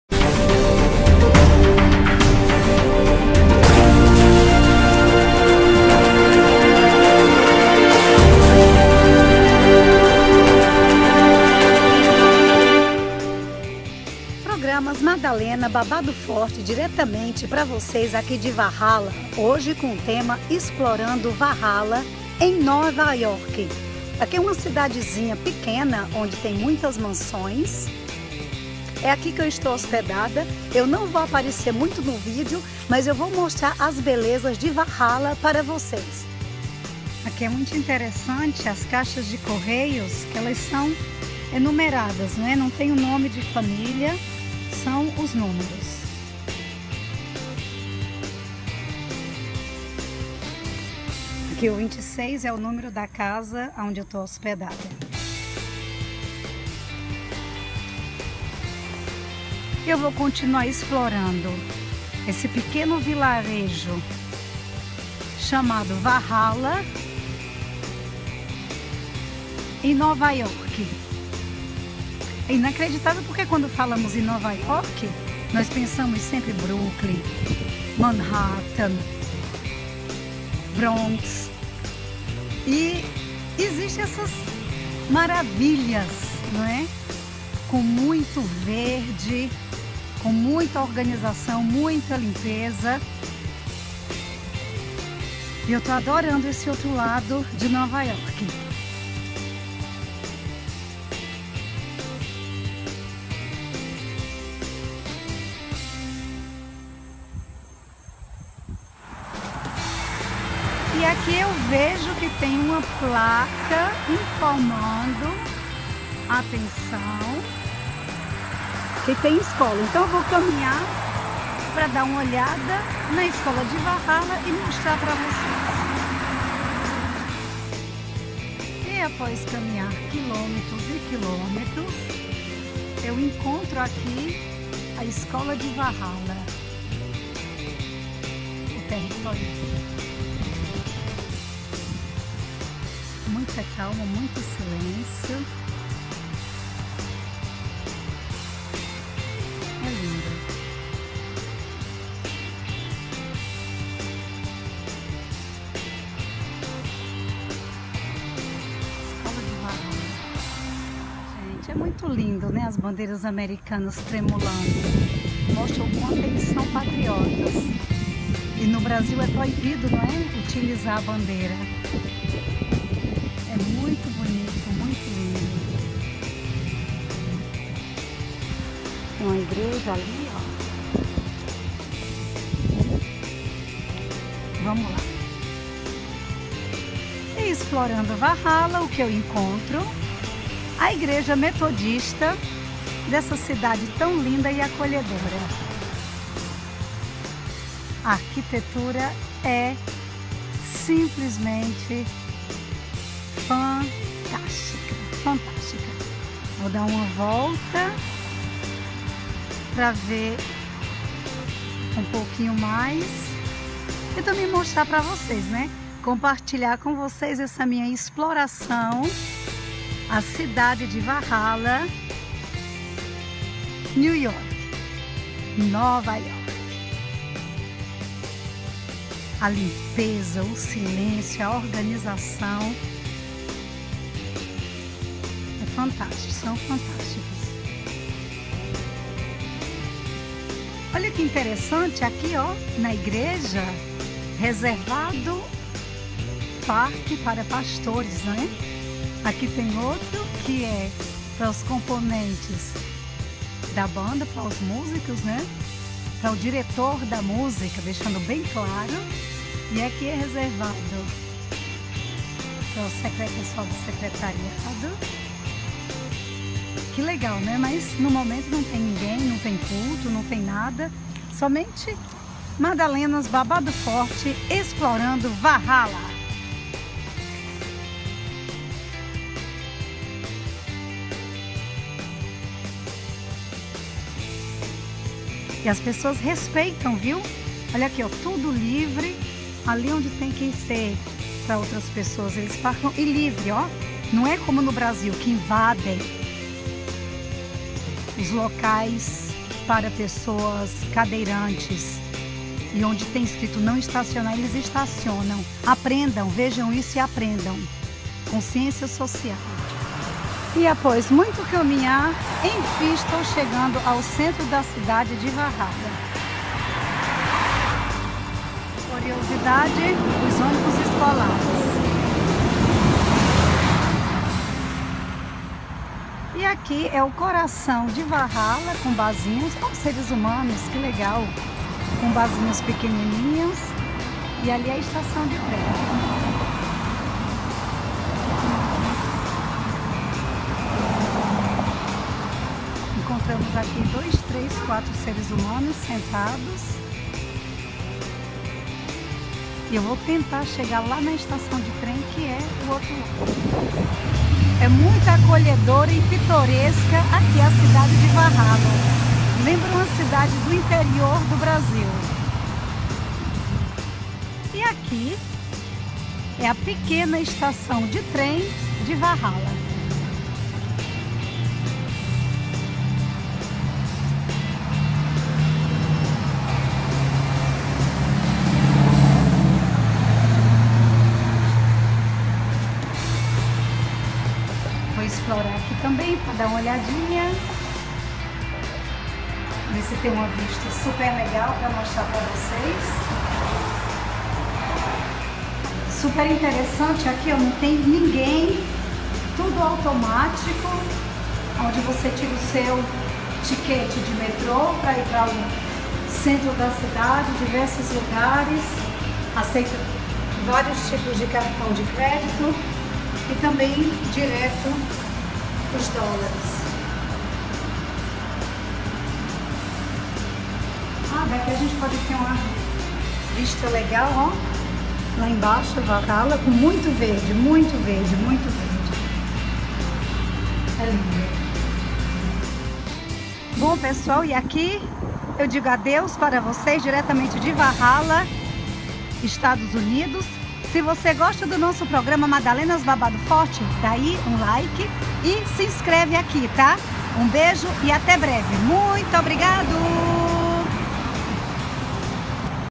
Explorando Valhalla-New York
Um pitoresco vilarejo localizado no Condado de Wester Chester em Nova York